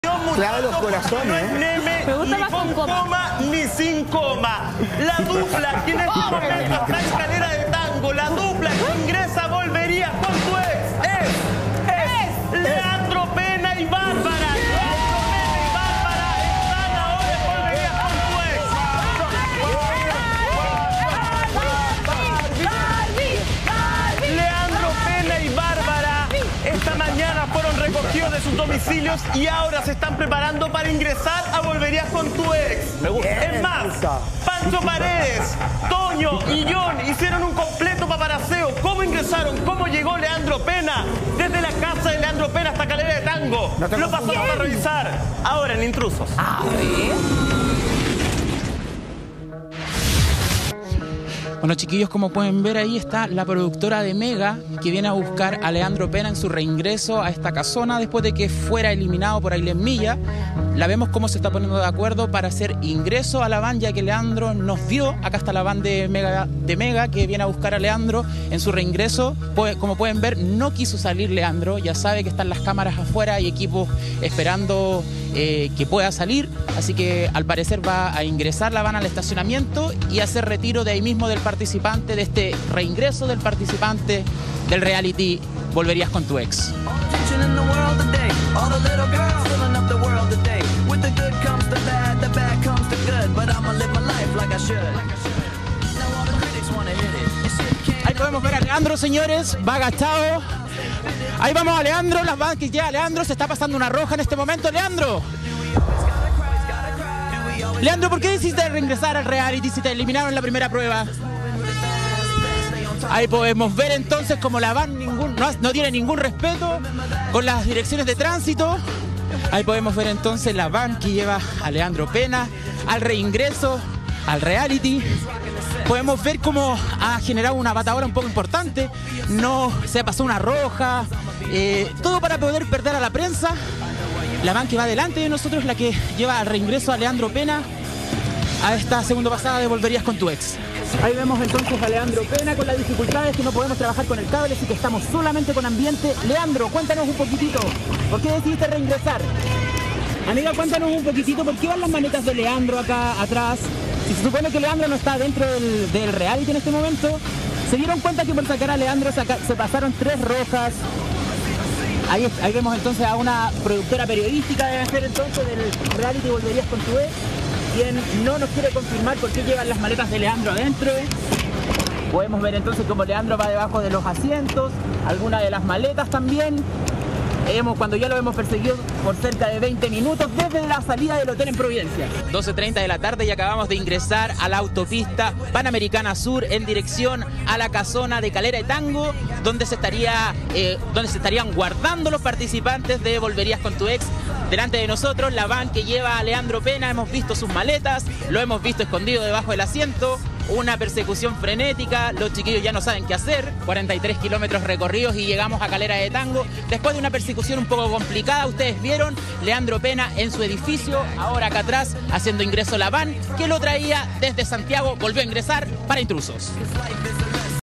¡Exclusivo! te contamos quienes reingresa al reality
La dupla que volvió a encerrase esta mañana en la casona de Calera de Tango está compuesta por Leandro Penna y Bárbara  Córdova.